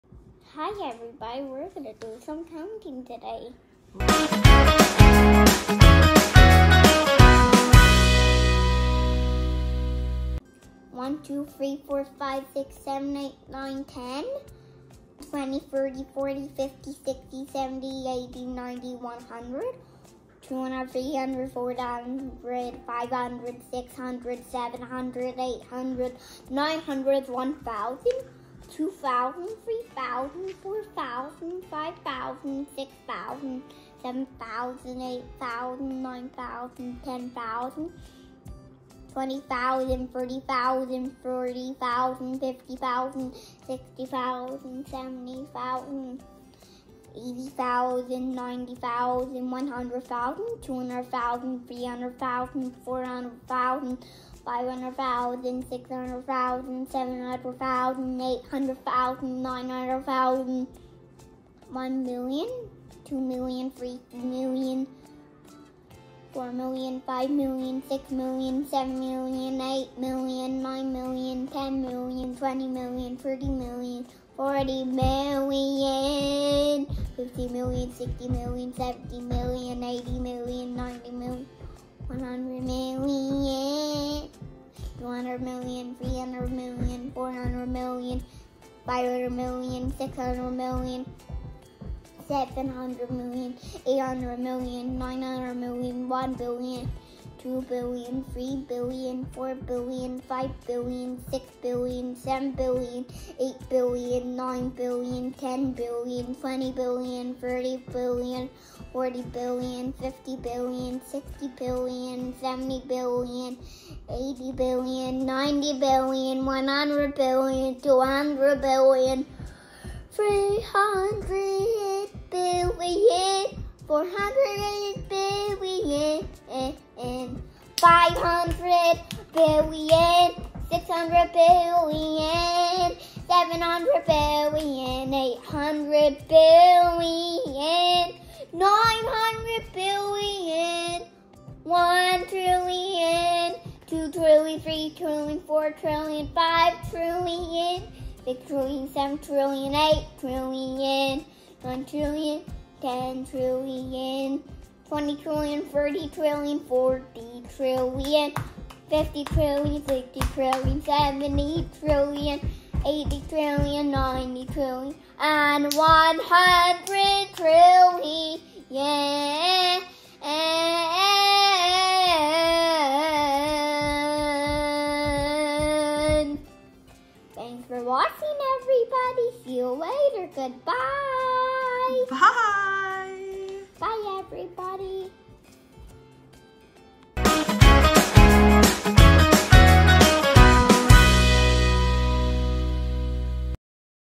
Four Year Old Emmett Counts To 100 Trillion In A Fun Way
Emmett's Space

In this video, you'll witness four year old Emmett count by place values to a large number: 100 trillion! That's 100,000,000,000,000 in numerical form, and it's a number so large that it's hard to even fathom.

But Emmett is up for the challenge! As you watch the video, you'll see him count confidently, moving from one number to the next. You’ll see how focused and determined he is to get to 100 trillion.

This video is not only entertaining, but it's also a great way to teach children about numbers and counting. By watching Emmett count to such a huge number, kids can begin to understand the magnitude of numbers and how they relate to each other. Plus, it's just fun to see someone count to such an enormous number!

So sit back, relax, and prepare to be amazed as you watch Emmett count to 100 trillion using place values. Who knows, maybe you'll even be inspired to try counting to a huge number yourself!